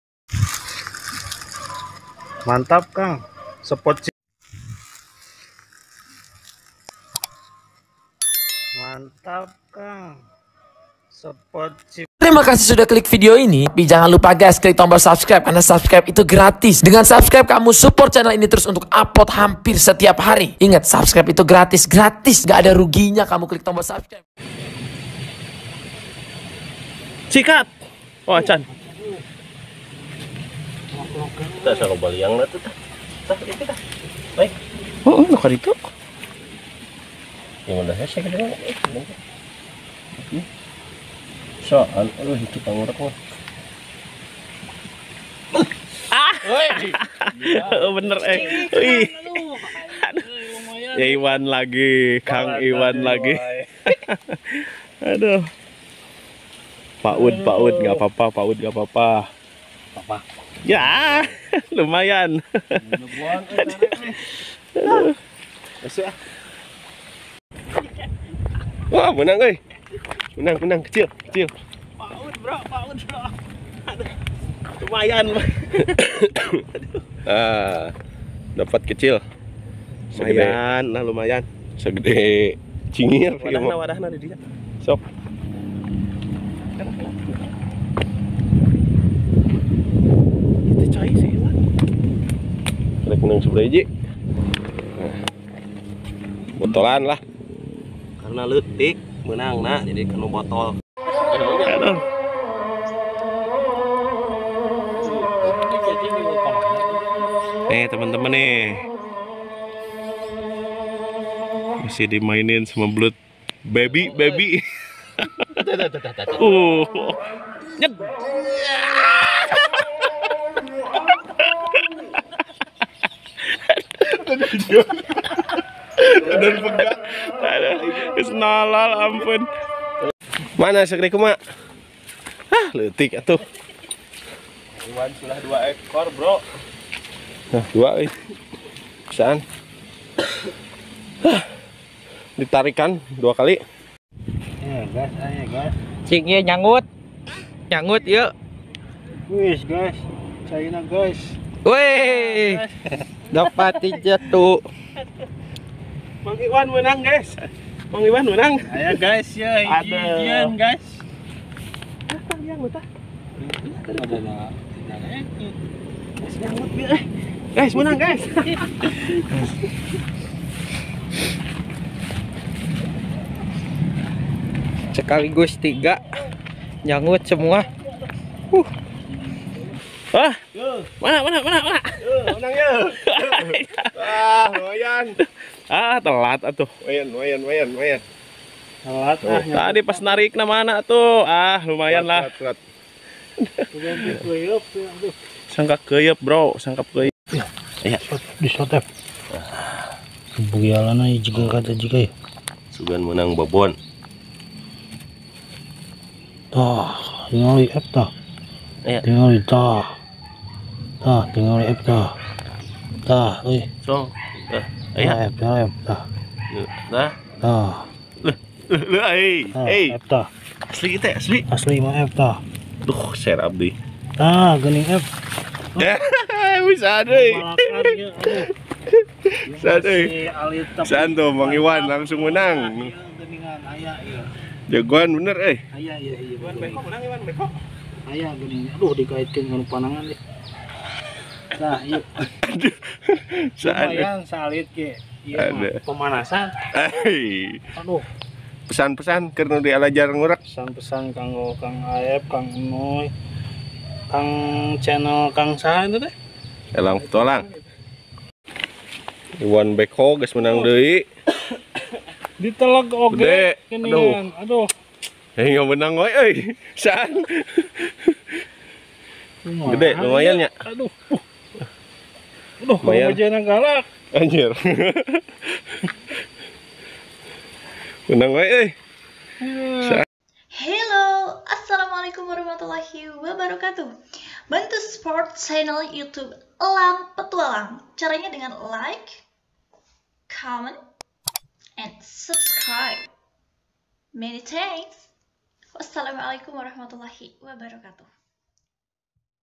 Kompilasi best moments mancing belut ELang Petualang
Best moments sangutan-sangutan saat mancing belut,luarbiasa seru mari dapetkan babon nya,dan siapakah juaranya,tonton video nya sampe selesai.